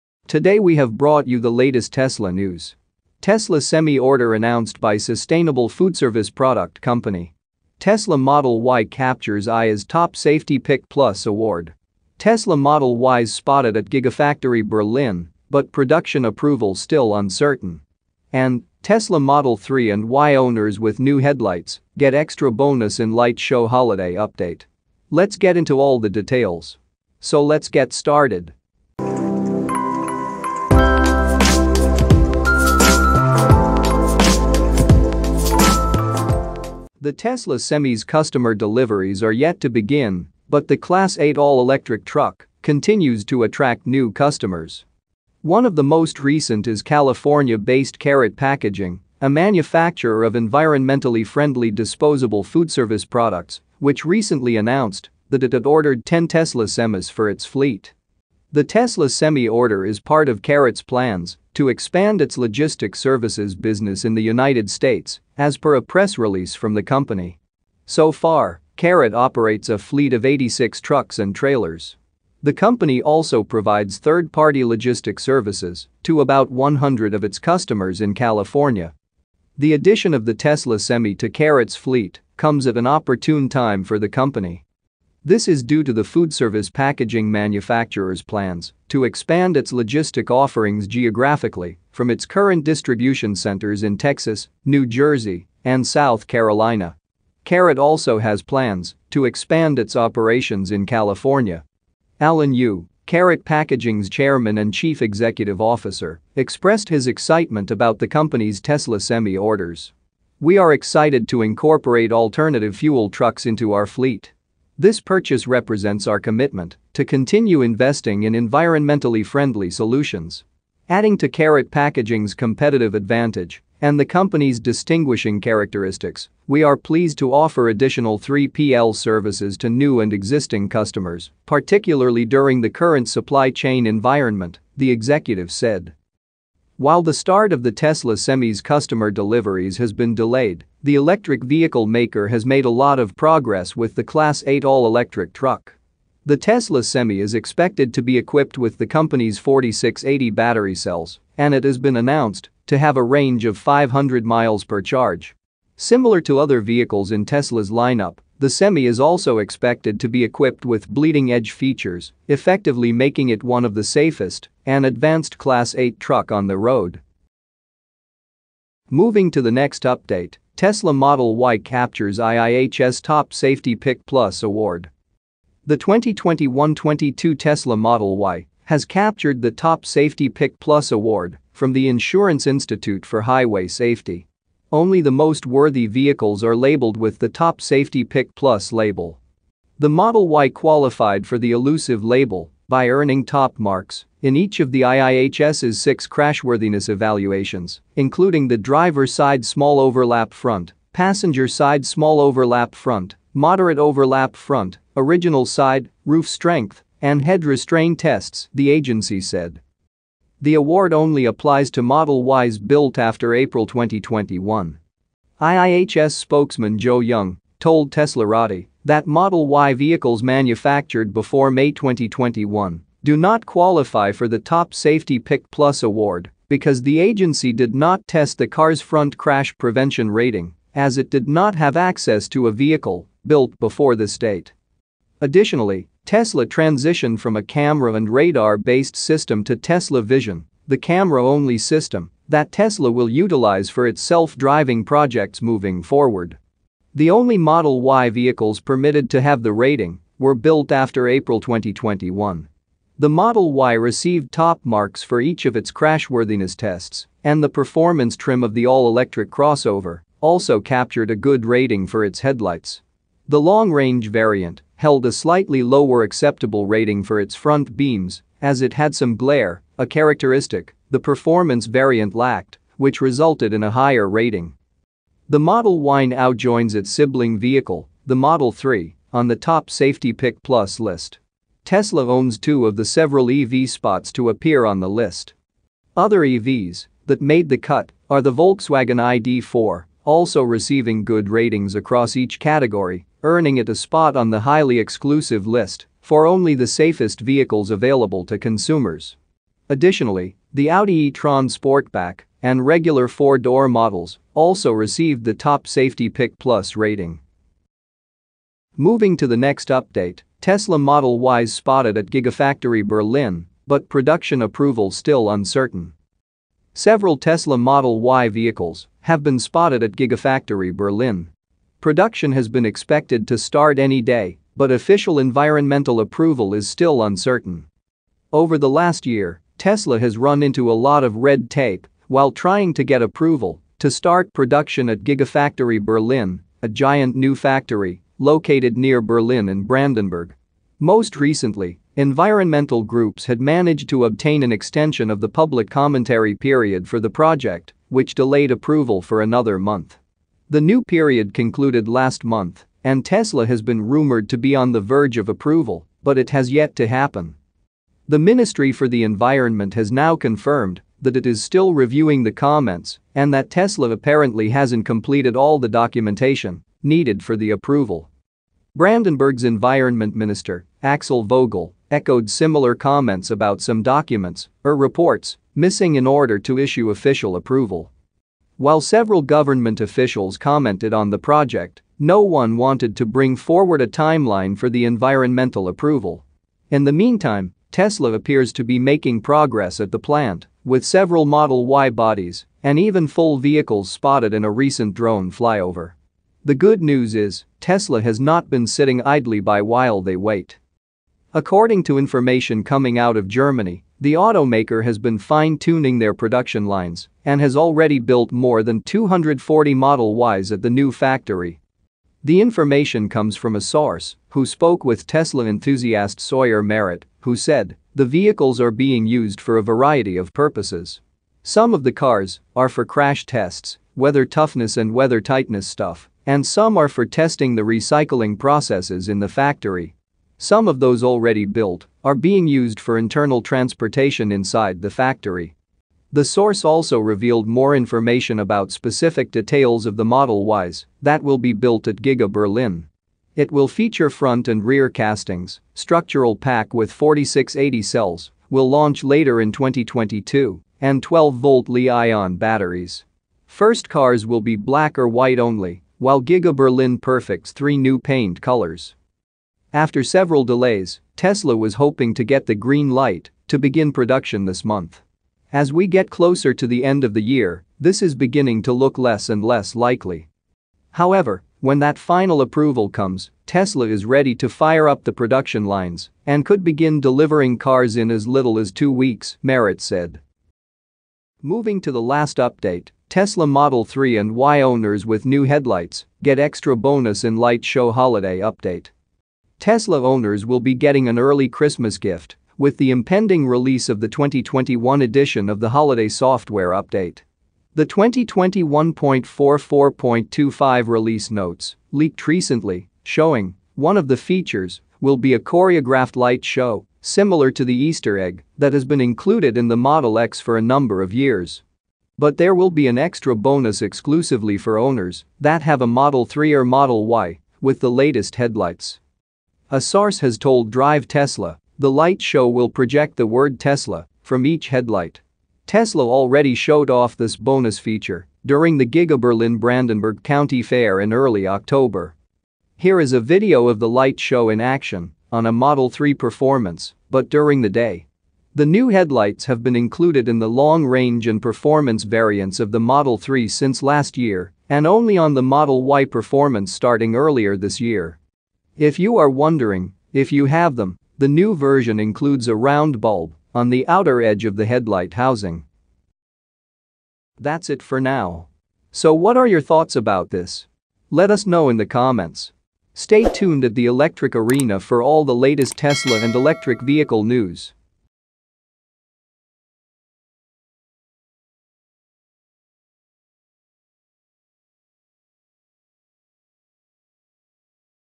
Tesla's New Model Y 2.0 Spotted & More Updates!
Tesla Semi order announced by sustainable foodservice product company
Tesla Model Y captures IIHS Top Safety Pick+ award
Tesla Model Ys spotted at Gigafactory Berlin, but production approval still uncertain
Tesla Model 3/Y owners with new headlights get extra bonus in Light Show holiday update